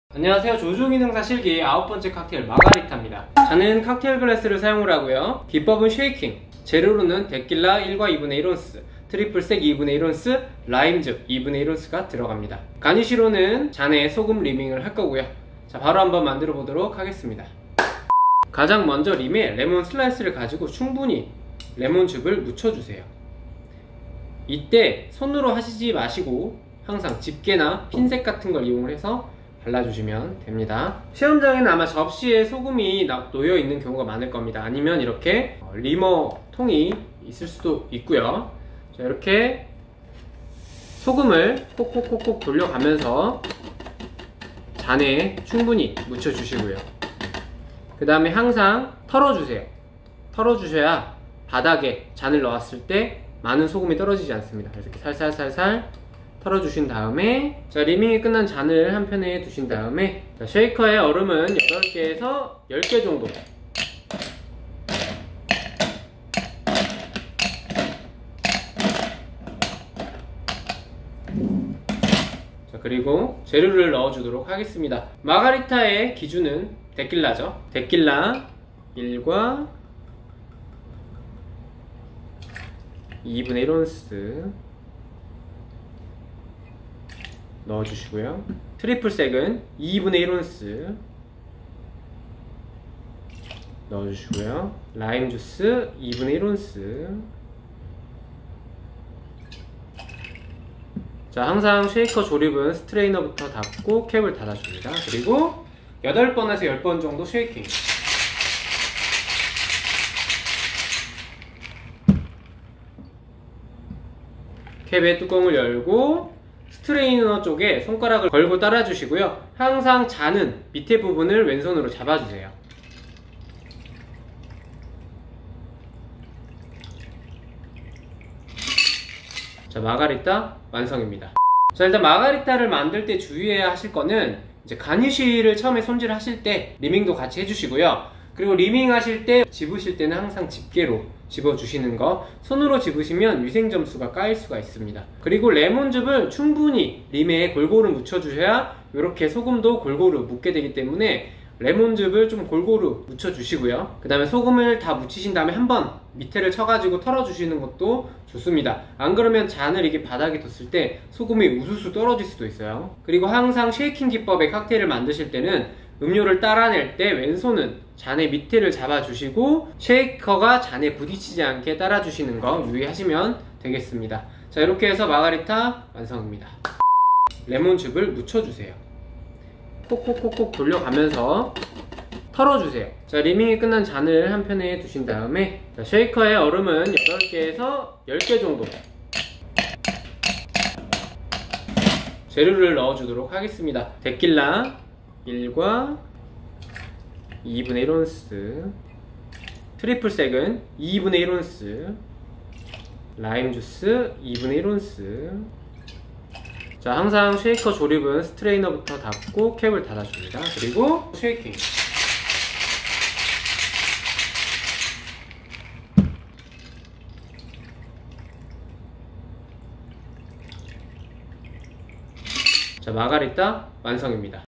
조주기능사 실기 09 마가리타 Margarita 만드는 법 & Tip
09 마가리타 Margarita

글라스 Cocktail Glass

기  법 Shake

재  료 1 Tequila 1 1/2oz
       2 Triple Sec 1/2oz
       3 Lime Juice 1/2oz

가니쉬 Rimming with Salt

레시피

1. 레몬 슬라이스로 칵테일 글라스 림에 레몬즙을 바른다.
2. 레몬즙이 묻은 림에 소금을 묻힌다.
3. 쉐이커에 얼음은 8~10개 정도 넣는다.
4. 테킬라 1 1/2oz, 트리플 섹 1/2oz, 라임주스 1/2oz를 넣는다.
5. 스트레이너, 캡 순으로 닫고 8~10회 흔든다.
6. 왼손으로 잔의 밑부분을 잡고 따른다.

* 마가리타, 키스 오브 파이어의 조주 레시피가 변경되었습니다. 칠링이 추가됨에 따라 변경된 조주법에 따라 추후 영상 업로드 및 레시피 공유드리겠습니다.

수정 레시피
1. 잔에 얼음을 넣고 칠링한다.
2. 쉐이커에 얼음을 8~10개 정도 넣는다.
3. 테킬라 1 1/2oz, 트리플 섹 1/2oz, 라임주스 1/2oz를 넣는다.
4. 스트레이너, 캡 순으로 닫고 8~10회 흔든다.
5. 칠링된 얼음은 버리고 레몬 슬라이스로 칵테일 글라스 림에 레몬즙을 바른다.
6. 레몬즙이 묻은 림에 소금을 묻힌다.
7. 왼손으로 잔의 밑부분을 잡고 칵테일을 따른다.

술기로운 생활 보내세요~